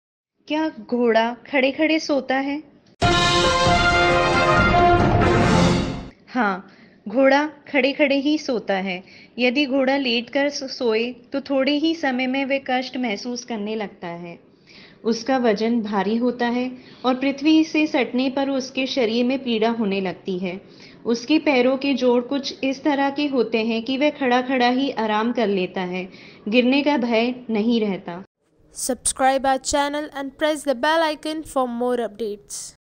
Incredible Facts: Why does a Horse Stand Asleep?? || आखिर क्यों सोता है घोड़ा खड़े खड़े
How Horses Sleep while Standing Up ! | घोड़ा खड़े खड़े ही कर लेता है आराम